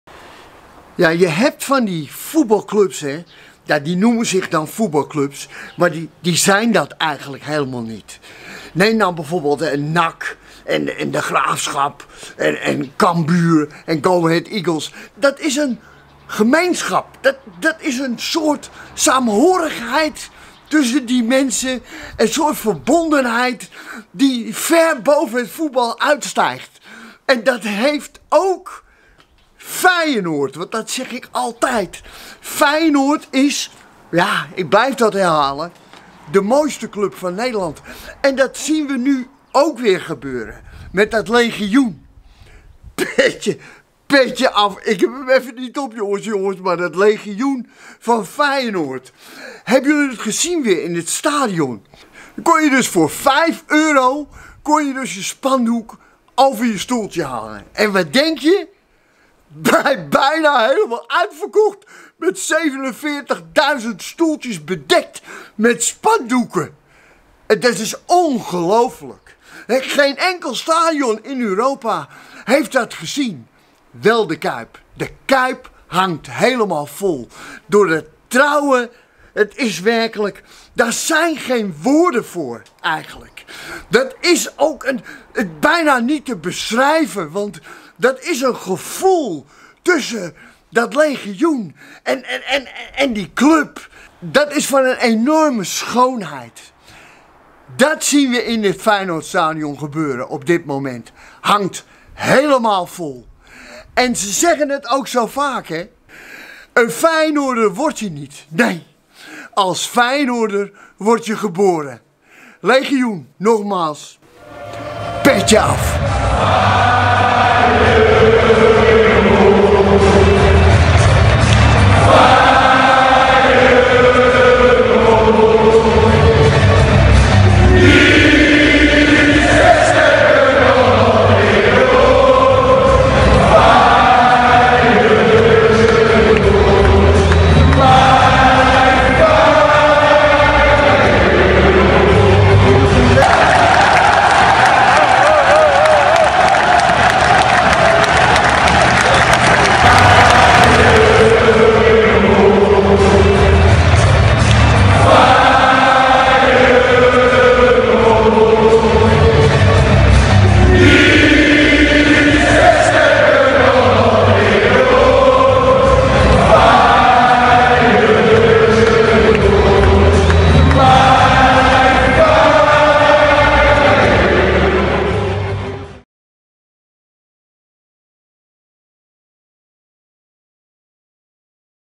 Spandoeken in De Kuip!
Petje af voor het legioen! De Kuip hangt vol met spandoeken. Het bewijs dat Feyenoord meer is dan een voetbalclub!!